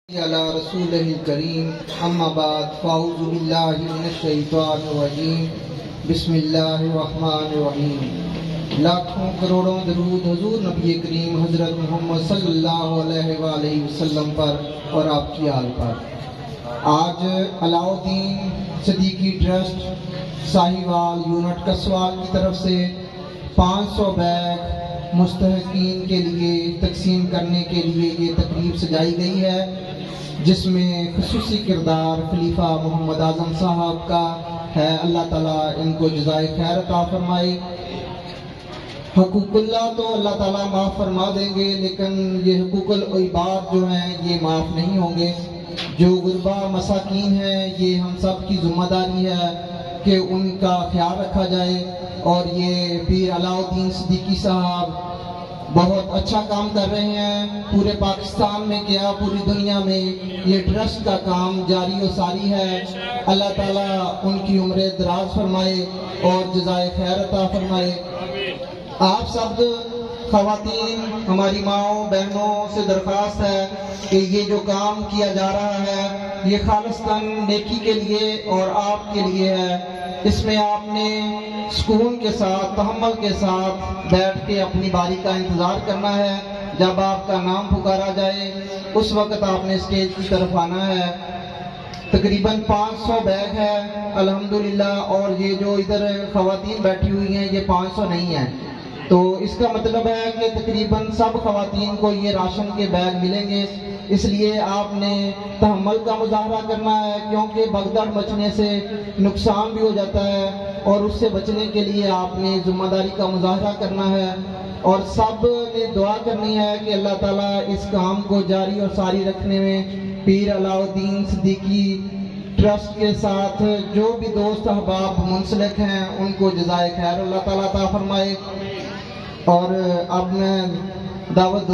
Alauddin Siddiqui Trust Ramzan Food Projects
Alauddin Siddiqui Trust Ramadan 2023 Food Packs distribution 📦
Location : Kassowal, District Sahiwal, Punjab, Pakistan.
‎الحَمْد للّٰهْ علاٶالدین صدیقی ٹرسٹ کے زیر اھتمام اور جانشین حضور شیخ العالم پیر محمد نور العارفین صاحب دامت برکاتہم العالیہ سجادہ نشین دربار عالیہ نیریاں شریف کی خصوصی ہدایات پر کسوال، ضلع ساہیوال کے مستحقین میں رمضان فوڈ پیکس تقسیم کیے گئے۔
‎زیرنگرانی: خلیفہ پیر محمد اعظم نقشبندی صدیقی صاحب
‎علاٶالدین صدیقی ٹرسٹ ساہیوال 💞

‎ فیضان صدیقی جاری رھے گا إن شاءاللّٰه عزوجل🌹

Watch daily live Alauddin Siddiqui Trust Appeals from 6pm on the Noor TV.

Jazak Allah